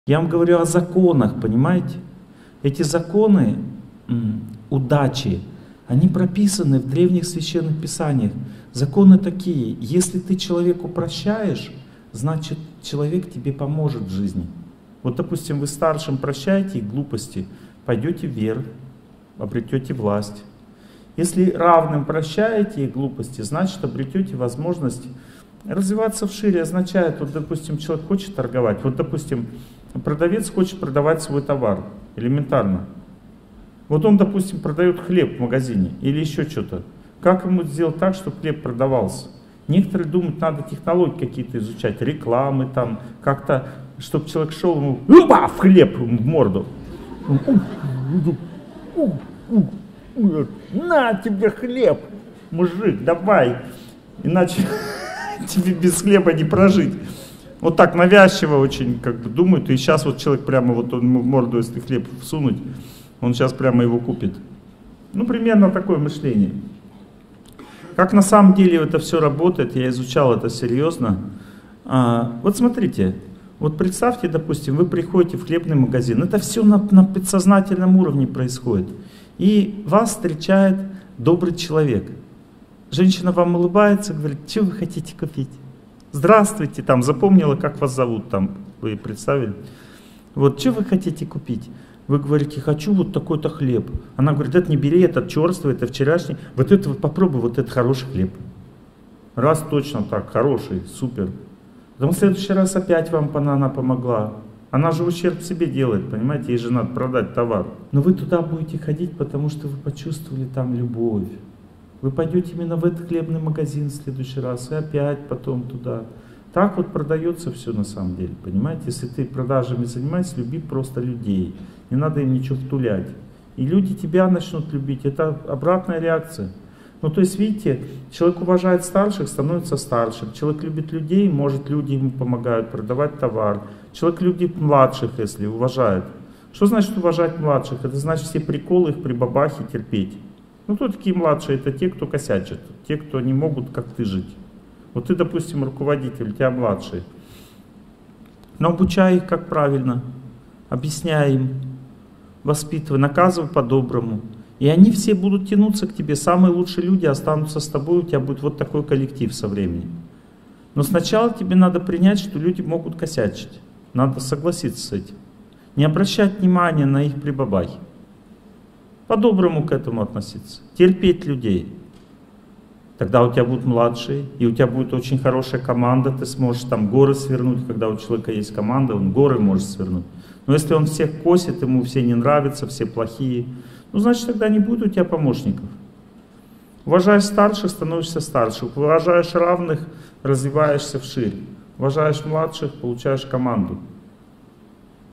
Торсунов О.Г. Законы удачи
В этом видео Олег Геннадьевич Торсунов говорит о законах удачи
Из лекции «Чувство собственного достоинства. Понимание себя и достижение успеха»,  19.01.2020 Одесса